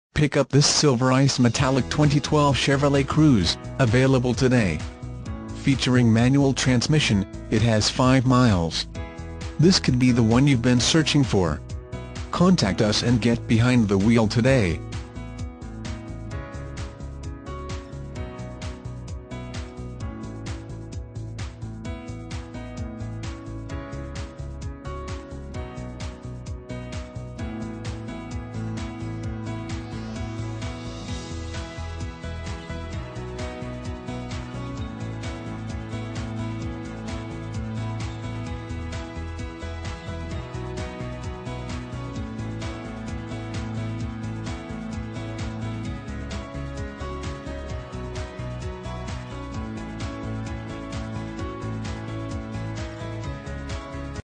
2012 CHEVROLET CRUZE Austin, TX C7404731
2012 CHEVROLET CRUZE Austin, TX
Stock# C7404731

Henna Chevrolet
8805 North IH-35

Pick up this Silver Ice Metallic 2012 Chevrolet Cruze, available today. Featuring Manual transmission, it has 5 miles. This could be the one you've been searching for.